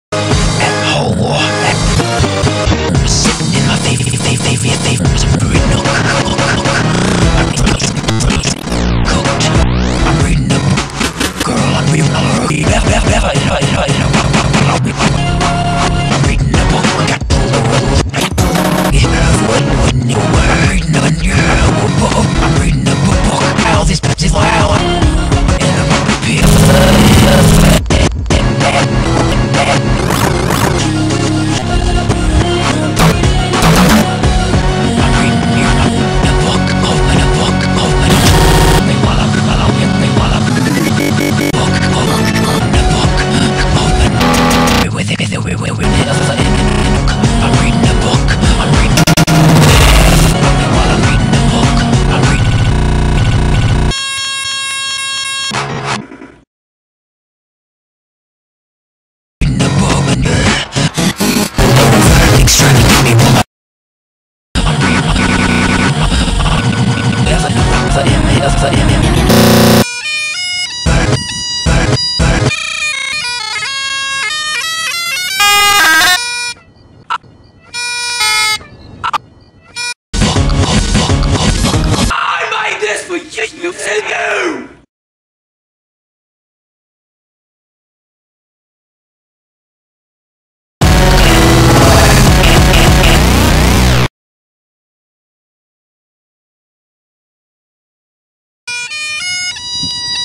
Julian Smith's Definite Bookjab.APV
.Veg replace from one of 1Superchops' videos. Check out his stuff if you haven't yet, he's got some great stuff. Also filler while putting together the classic YTPMV collab.